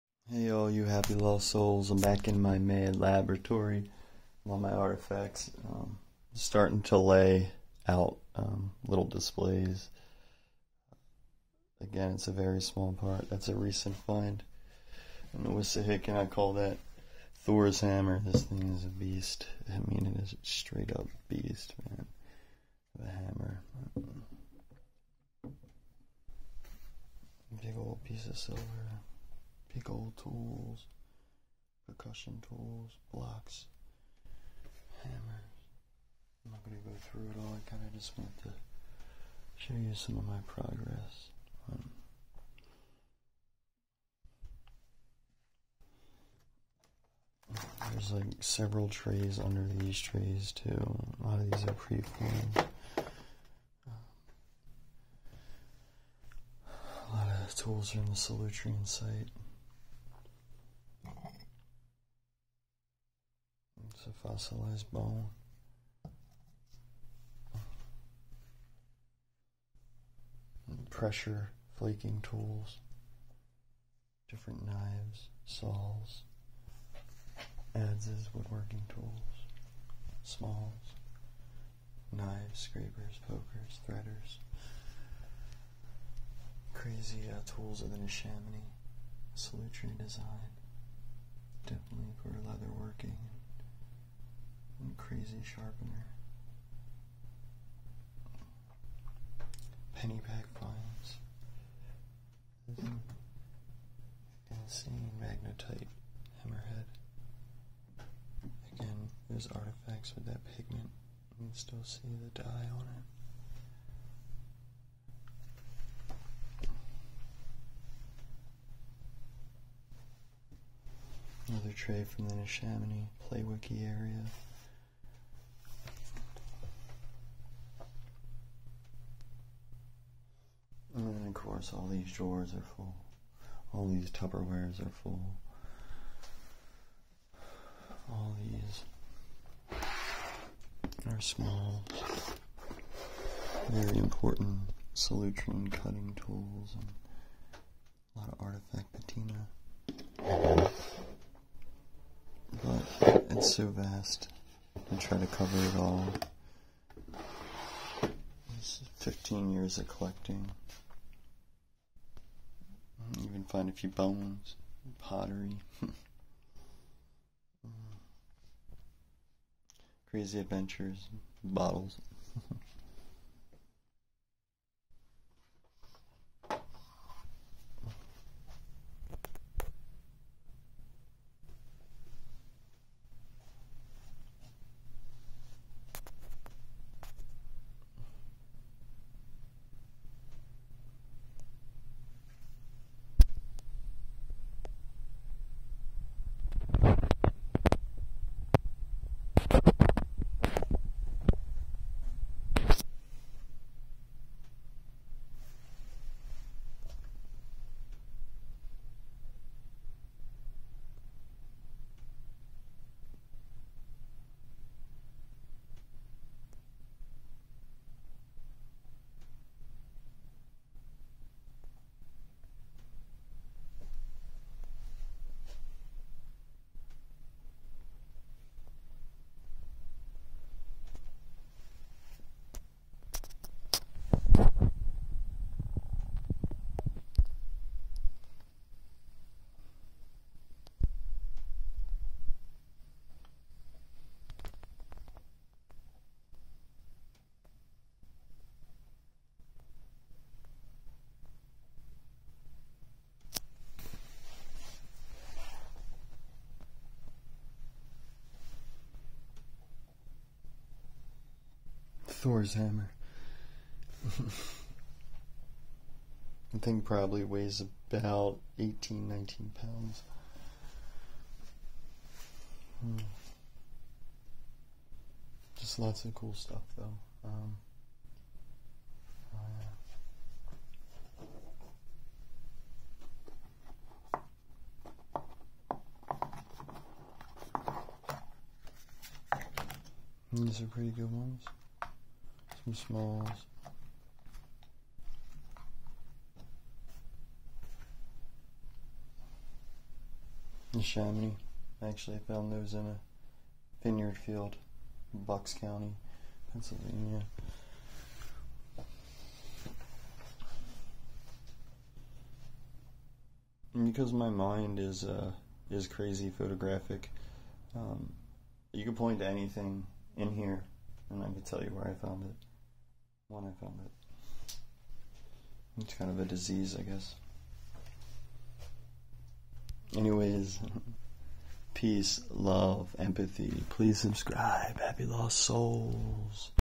Crazy Stone Tools Found in South Eastern Pennsylvania!! Pre-Clovis!!!! Rethinking ancient America!!
a low quality quick lil tour been organizing my stone tool collection.. All these finds come from the Pennypack watershed, the Wissahickon watershed, and The Neshaminy watershed. the finds go back possibly 60k years or more up to far more recent finds colonial...please subscribe!